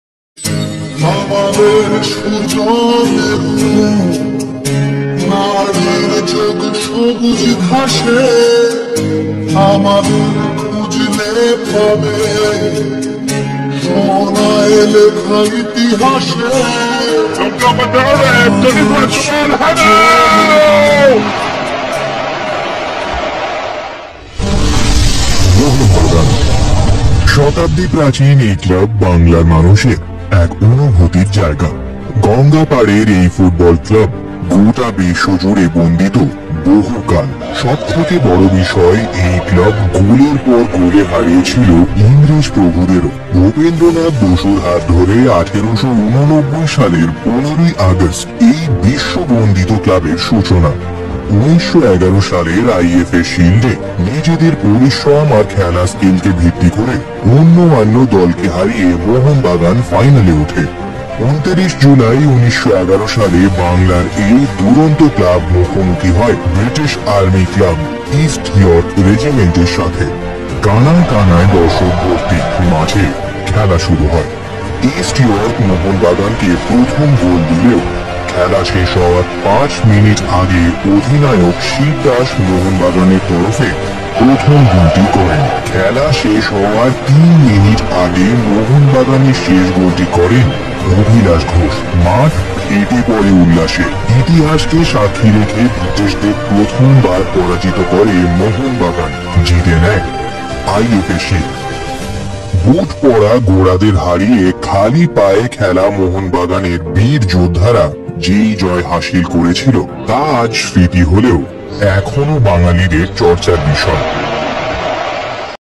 ইংরেজদের বিরুদ্ধে মোহনবাগানের সেই ঐতিহাসিক জয়ের মুহূর্তের বিবরণ!💚❤️২৯শে জুলাই
২৯ শে জুলাই শুধু একটি তারিখ নয়, বাঙালির স্বাধীনতা সংগ্রামের ইতিহাস চিরস্মরণীয় একটি দিন, ১৯১১ সালের এই দিনেই IFA  শিল্ড ফাইনালে শিবদাস ভাদুড়ীর নেতৃত্বে সবুজ মেরুন জার্সি পরিহিত মোহনবাগানের ১১ জন বীর যোদ্ধার অদম্য ল ড়া ইয়ের কাছে হা র মানতে হয়েছিল প্রবল পরা ক্রমী ব্রিটিশশক্তির মূত প্রতীক ইয়কশায়ার রেজিমেন্টকে